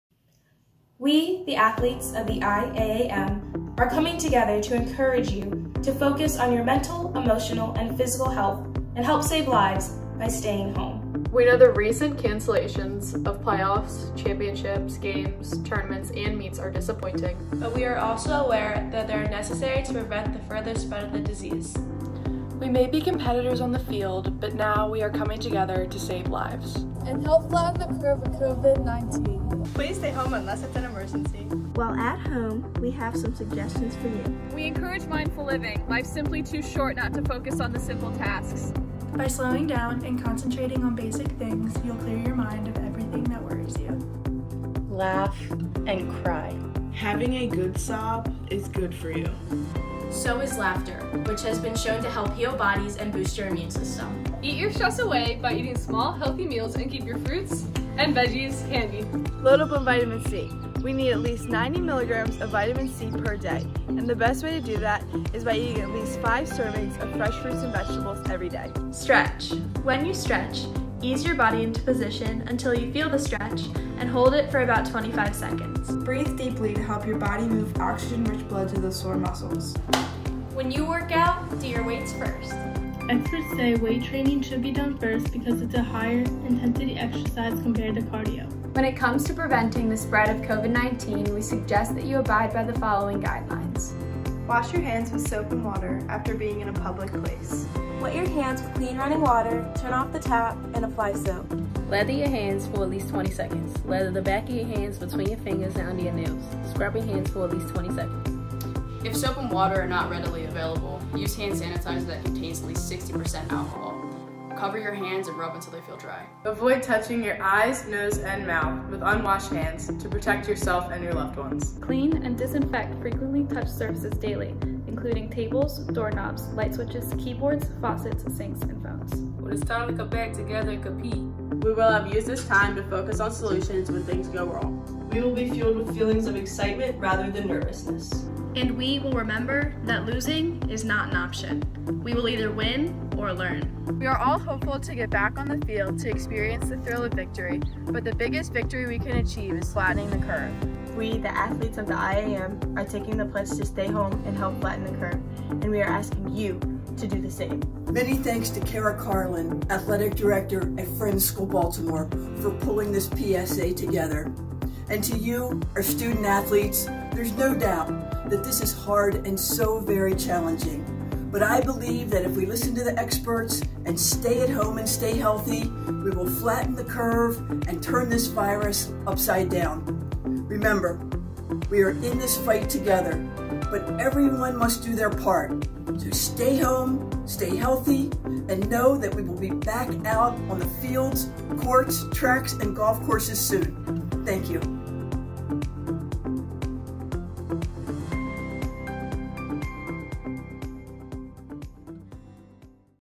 I.A.A.M Covid-19 PSA 2020
This idea was "borrowed" from a high school league in Connecticut and theirs was a great inspiration. Hats off to the I.A.A.M and Athletic Director, Kara Carlin (Friends School) for spearheading this effort and leading the way. The league is pushing this PSA out to their communities and we are very proud of our efforts. This is a simple way to show that rivals on the field, can come together off the field in a time of need.